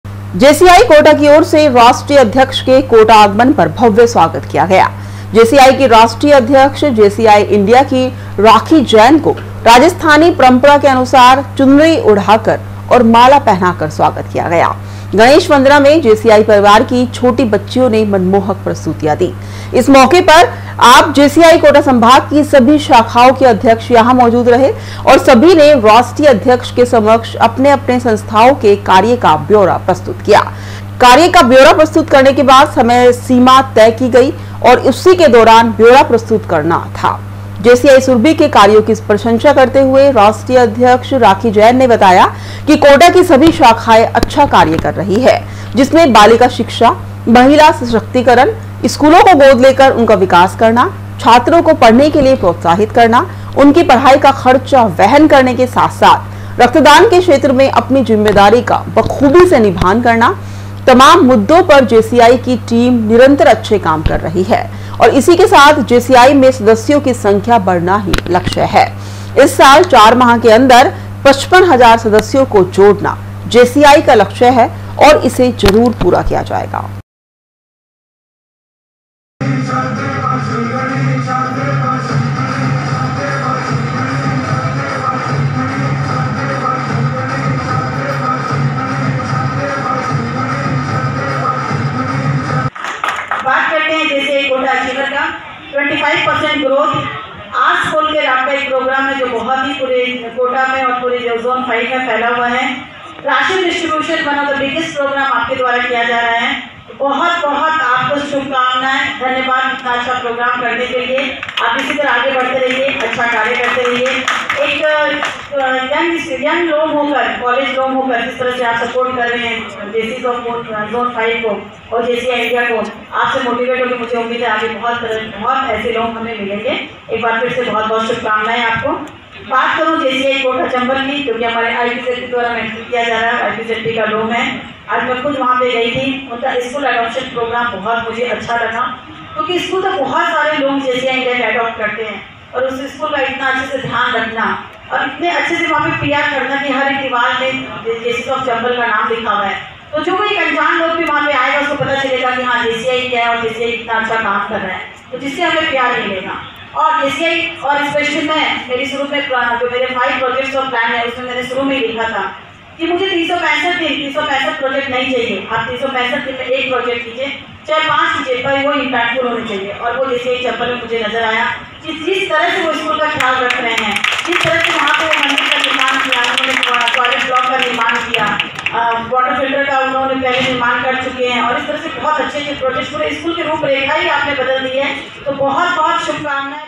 24 sep.2021 @news कोटा - जेसीआई कोटा की ओर से राष्ट्रीय अध्यक्ष कोटा आगमन पर किया भव्य स्वागत
जेसीआई कोटा की ओर से राष्ट्रीय अध्यक्ष कोटा आगमन पर किया भव्य स्वागत